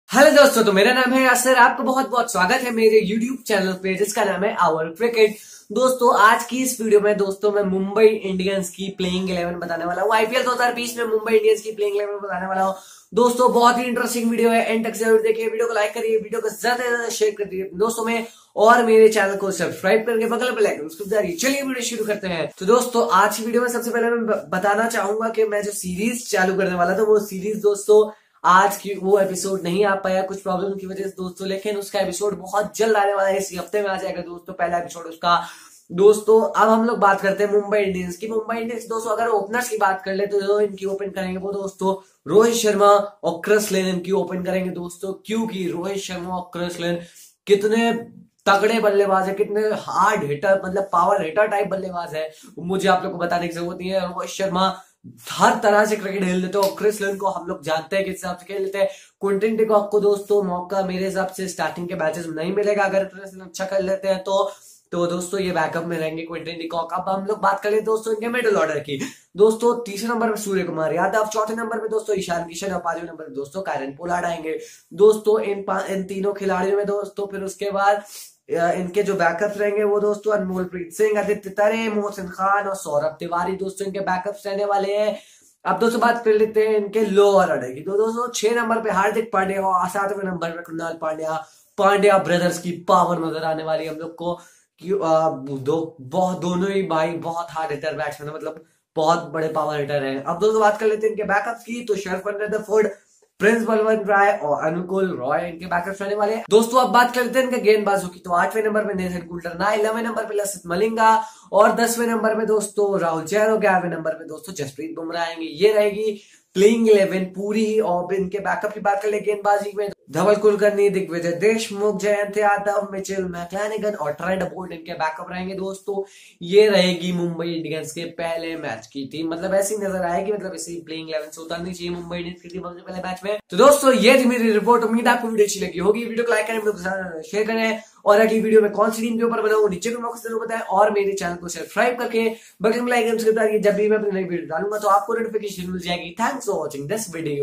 IPL 2020: MUMBAI INDIANS (MI) CONFIRMED PLAYING 11 IN IPL 2020 || OUR CRICKET
Iss video mein maine aap logon ko MUMBAI INDIANS ki Predicted playing 11 batai hai.Ummeed hai aapko video acchi lagegi.

About Host:
Name-SYED YASIR HUSAIN
Class-10th
Age-15
Lives in Lucknow